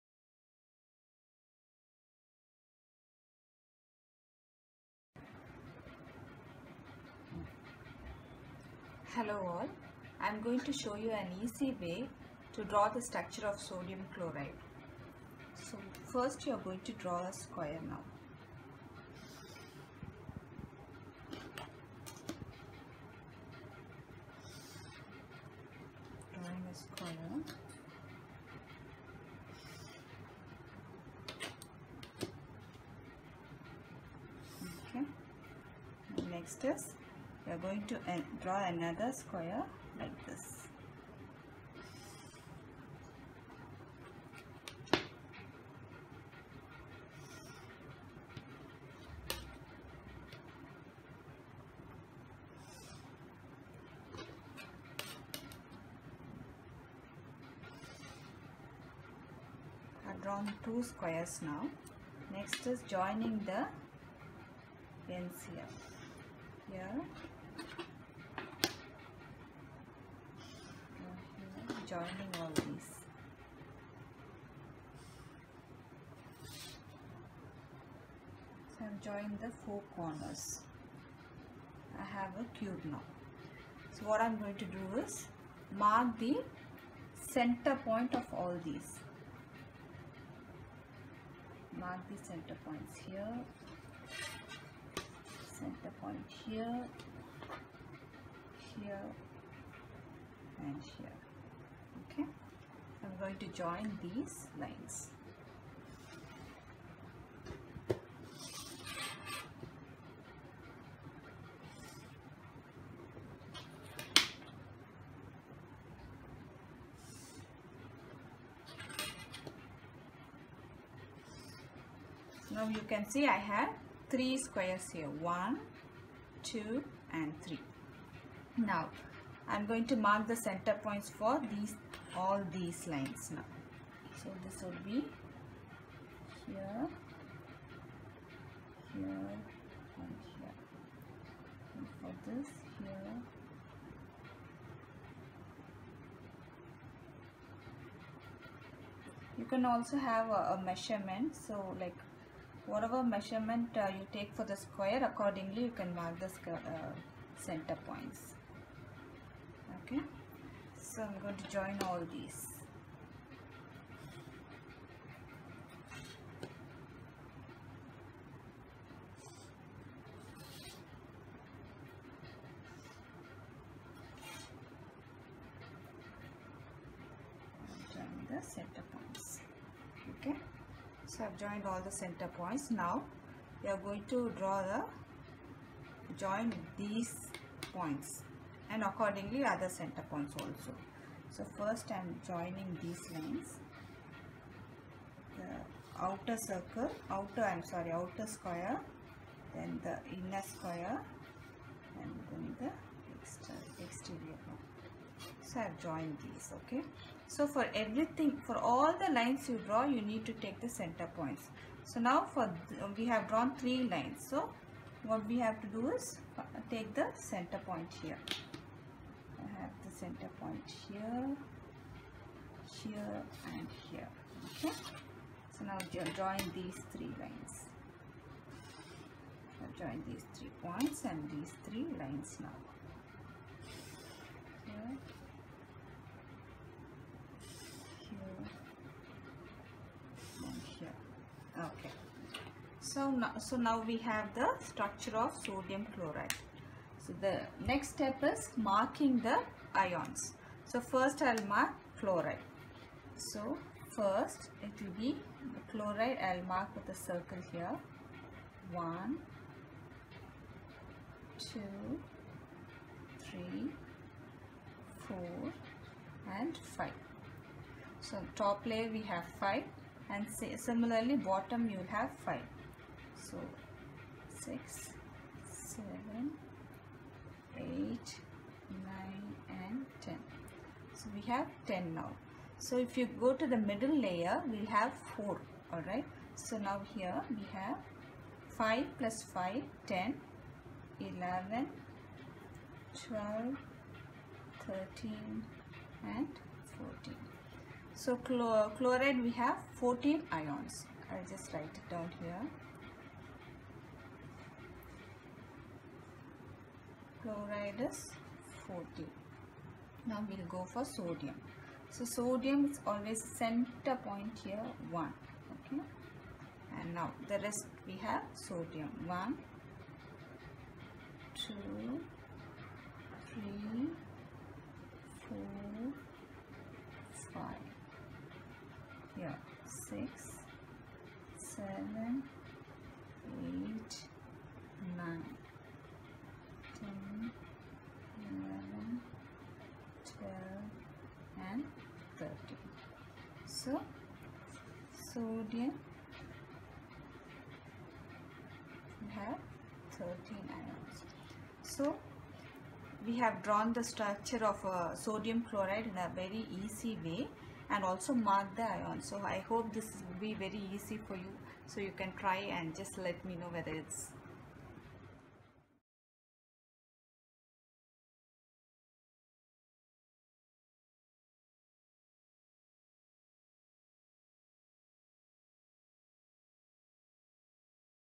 How To Draw Sodium Chloride (NaCl) Diagram?
Easy step-by-step guide to draw Sodium Chloride (NaCl) diagram.
Here is how to draw the Crystal Structure of Sodium Chloride (NaCl).